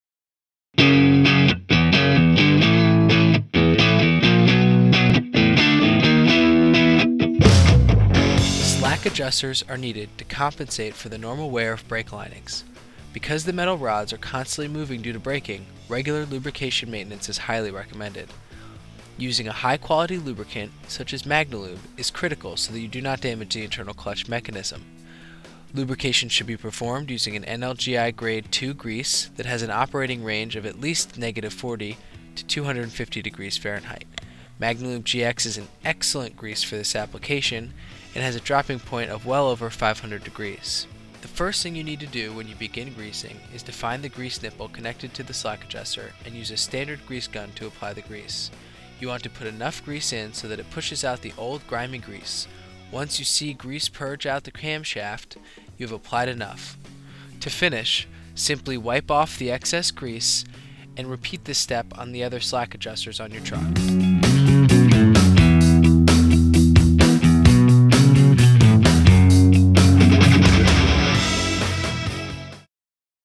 How To Grease Trucks: Greasing The Slack Adjusters with Magnalube-GX Extreme Pressure Grease
In this video, we demonstrate proper truck maintenance procedures by using Magnalube-GX PTFE grease with molybdenum disulfide (or moly) to grease the slack adjusters of a truck. Magnalube-GX is a lithium grease that performs very well as a grease for trucks because the MOLY-PTFE technology improves the ability to withstand extreme pressure and lower friction. It also has a lithium complex base which is compatible with other greases used to grease trucks. If you use Magnalube-GX grease for the slack adjusters and general truck maintenance, it will extend the life of your truck parts as well as decrease the amount of times that truck maintenance is required. Special thanks to Allstate Moving & Transfer Company of Linden, NJ.